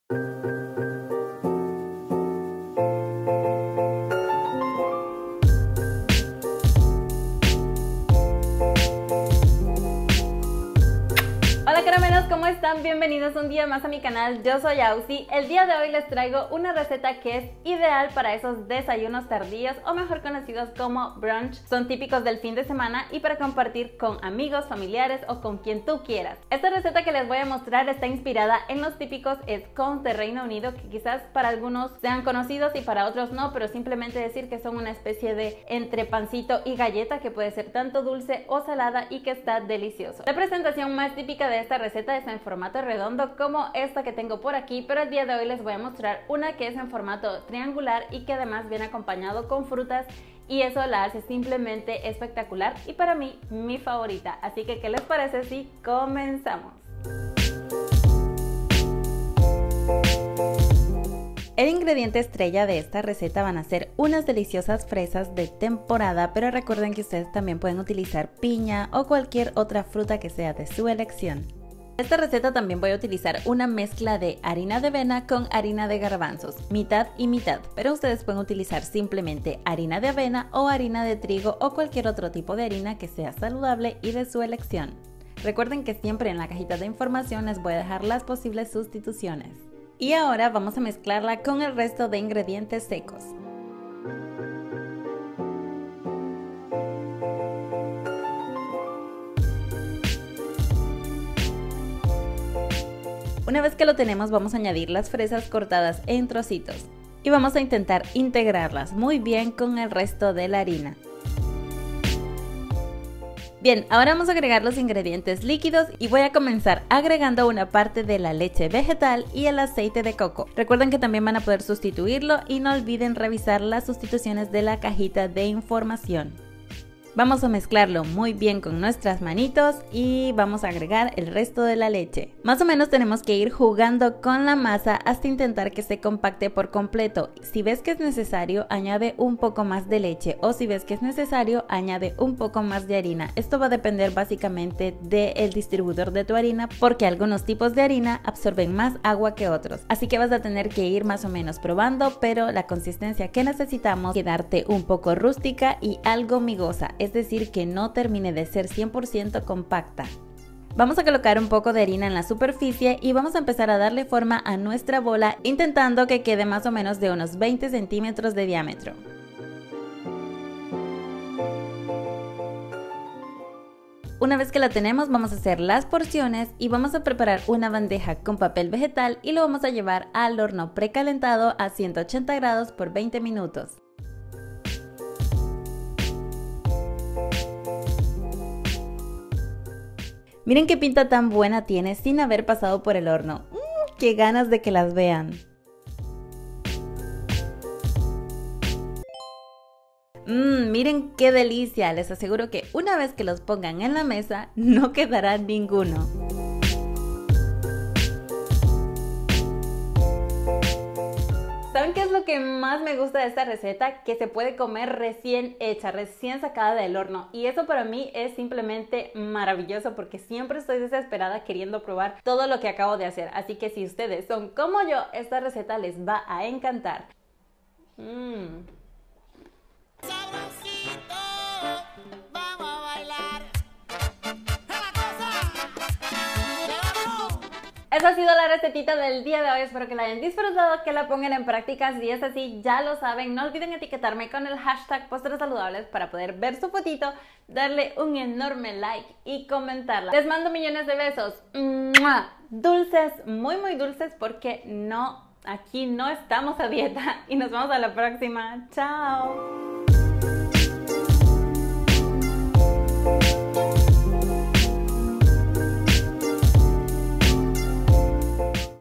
PASTELITOS de FREAS PARA DESAYUNAR | SIN HUEVOS NI LACTEOS | VG
❤  Descarga mis LIBROS (e-Books) de cocina saludables en tu móvil, tablet o laptop:

POSTRES SALUDABLES SIN HORNO
POSTRES SALUDABLES ESPECIAL FIESTAS
CENAS SALUDABLES GUÍA PRÁCTICA

Panela… qué es ?
Otros nombres:  Piloncillo, chancaca, papelón, Rapadura, Tapa dulce, Atado dulce, Panetela, Empanizao, Panocha, organic cane sugar, etc.  Verdad que la conoces!!

❤  RECETA
✍ Ingredientes  para 8 porciones pequeñas o 6 grandes | molde 20 cm de diámetro

220 g de harina de avena  ( o cómo hice yo: mitad harina de avena  + harina de Garbanzos )
4 cdas de Panela molida -azúcar de coco o de caña 100% integral-
¼ de cdita de extracto de stevia - sin stevia añadir 3 cdas más de panela-
1 cdita de vainilla
½ taza de leche vegetal
3 cdas de aceite de coco o mantequilla sin sal -no procesada-
1 cdita de Polvo para hornear
1 taza de fresas picaditas o la fruta que más te guste.

Horno precalentado a 180 ºc por 18- 20 min.

Info Nutricional:
1 pedazo de 8  =  180 kcal | Prot 4 g | Grasas 7 g | Carbs 23 g  * Este postre es una versión más ligera que la receta original.

*La información nutricional se proporciona sólo con el fin de crear conciencia sobre el valor nutricional de los alimentos. NO CUENTES CALORÍAS, CUENTA NUTRIENTES!

🌷  SUSTITUCIONES : dedito arriba si se te son útiles.

🍬 SugarFree - diabéticos, endulzar con stevia u otro edulcorante que sea resistente a las altas temperaturas del horno. al gusto.

🍂 VeganFriendly : Apta para veganos.
🌾 GlutenFree - Intolerantes al gluten usar avena CERTIFICADA sin gluten. Otras combinaciones, harina de arroz + harina de almendras (mitad y mitad) Harina de garbanzos + harina de almendras (mitad y mitad)

👉 Mis edulcorantes naturales favoritos + que es PANELA y todos sus nombres

Besitos Dulces... muy dulces